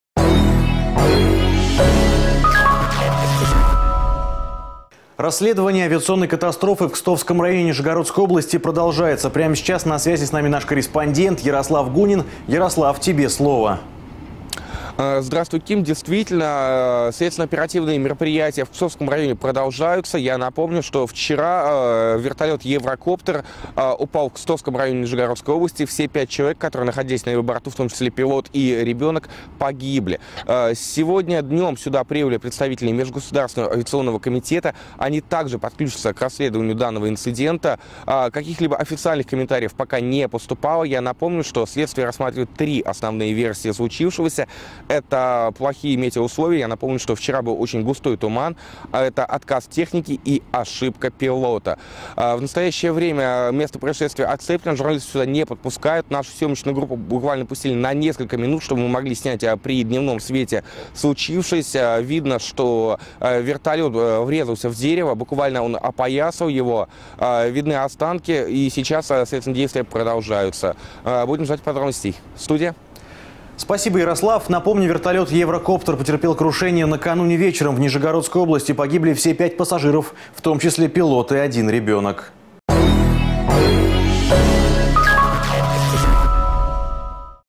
ПРЯМОЕ ВКЛЮЧЕНИЕ
Съемочной группе "Горький ТВ" удалось получить новые кадры с места крушения вертолета.